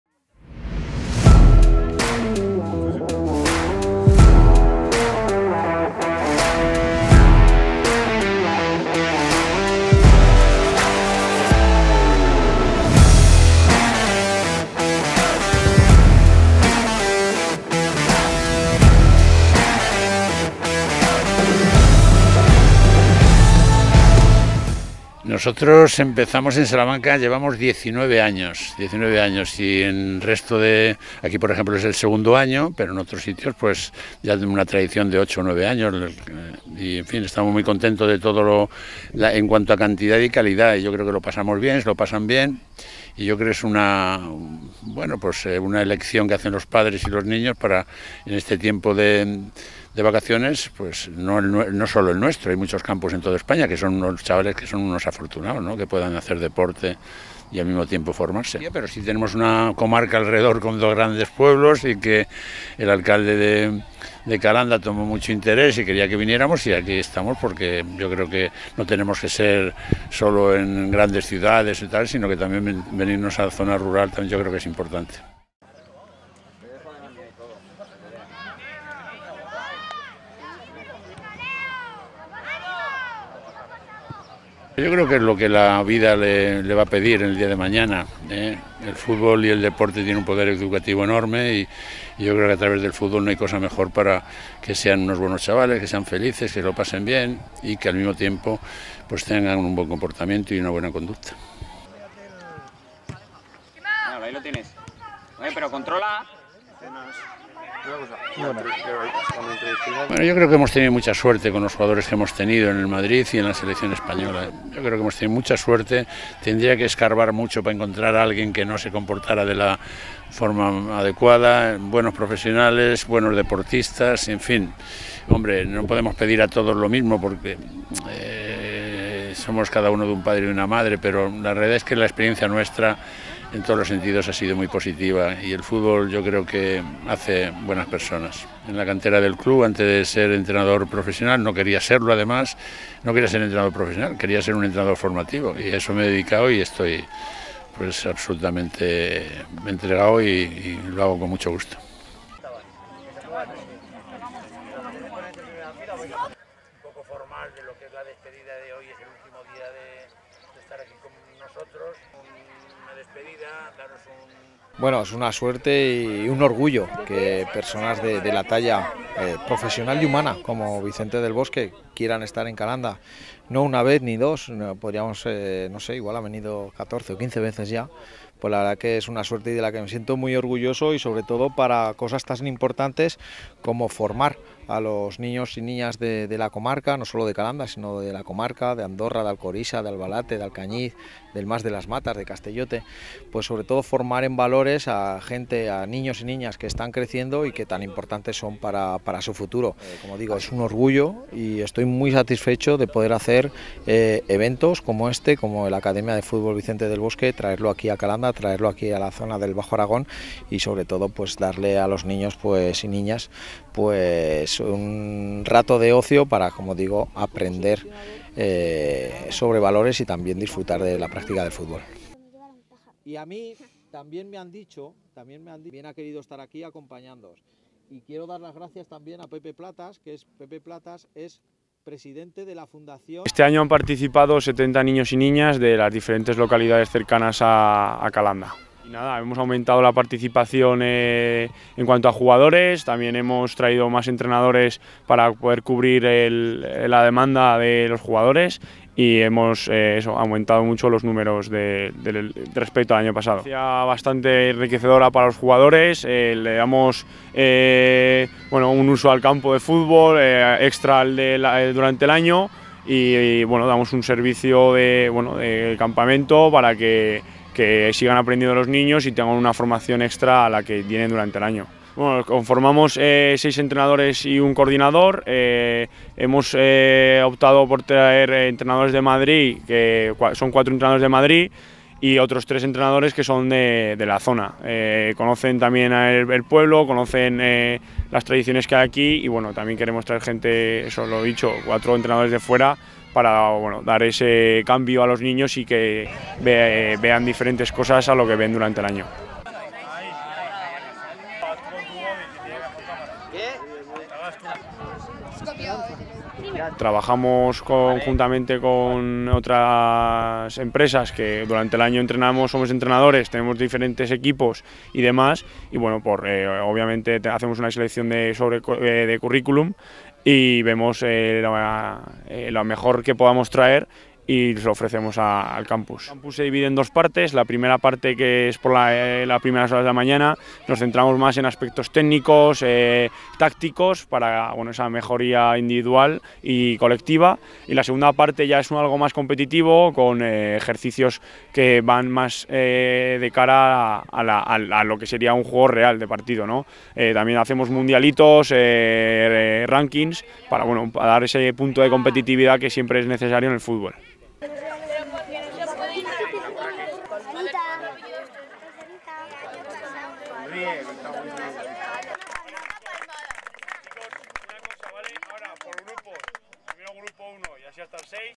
VICENTE DEL BOSQUE clausura la 2ª edición del campus FOOTBALL ACADEMY en CALANDA
Durante la mañana, protagonizada por el calor, Vicente del Bosque ha respondido alegremente a las preguntas de los jóvenes. Resguardados del sol, los niños han sorprendido han exfutbolista con sus conocimientos sobre partidos en los que «ni si quiera habíais nacido», además de sacarle alguna sonrisa con sus propias historias y hacerle reír con preguntas como «¿conoces a Ronaldo y Messi?».

Esta segunda edición en Calanda del campus de la Vicente del Bosque Football Academy ha contado con una charla del exseleccionador, quien ha repartido el diploma a cada uno de los chicos, que se acercaban nerviosos, y ha firmado todos los autógrafos necesarios sobre camisetas, mochilas, gorras e incluso, alguna bufanda de la Selección Española.